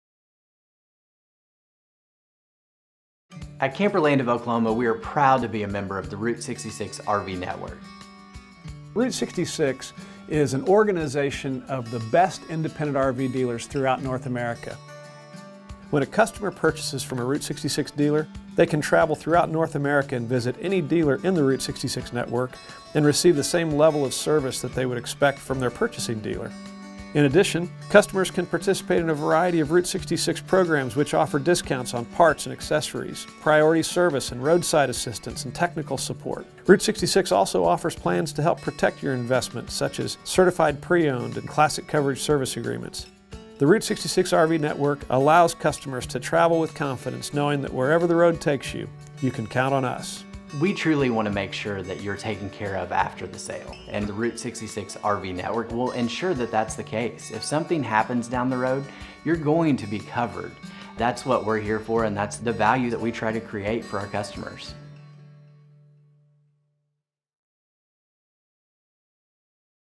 Camperland of Oklahoma - Route 66 RV Network
Camperland of Oklahoma is proud to be part of the Route 66 Network of RV Dealers. As a Camperland customer, you can be assured of hometown service, no matter where you travel.